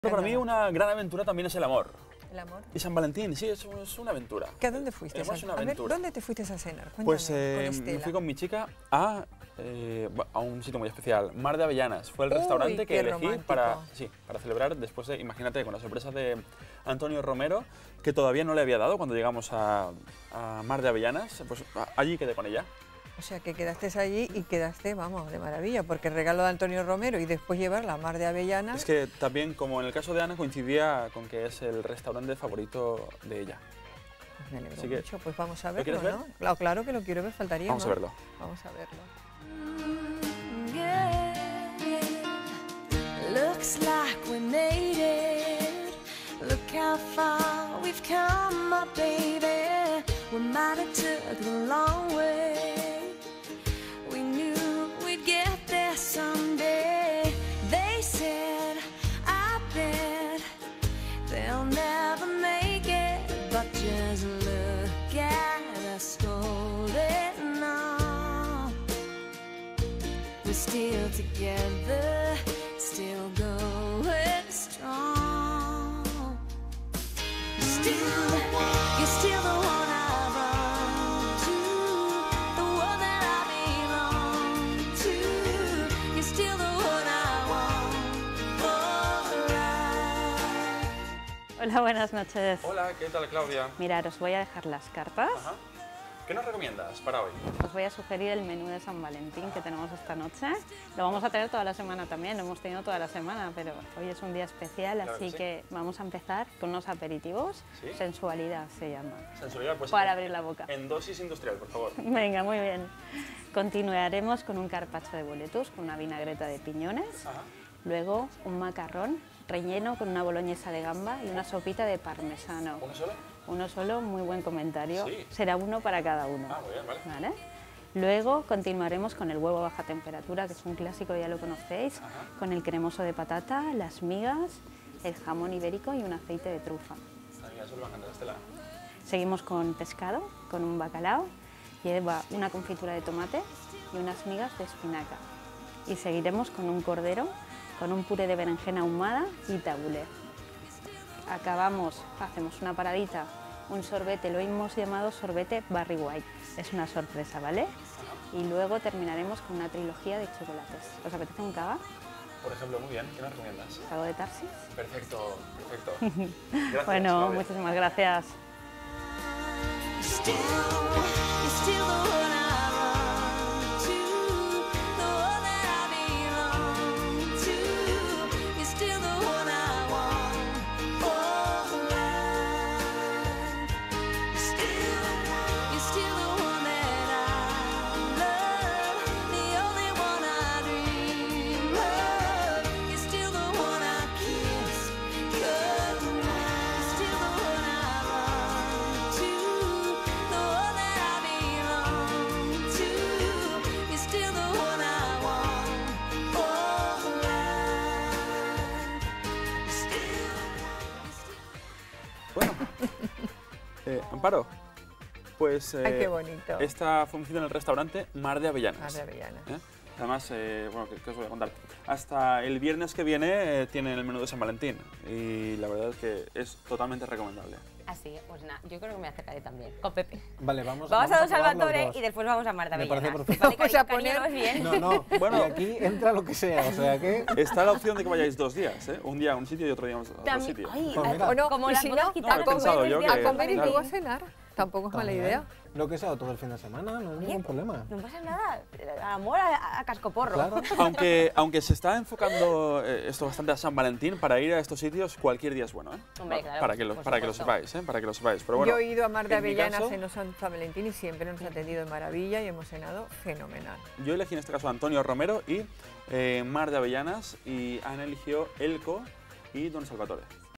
ESPECIAL SV: Donís escoge Mar de Avellanas para su cena romántica
Especial San Valentín
Programa 198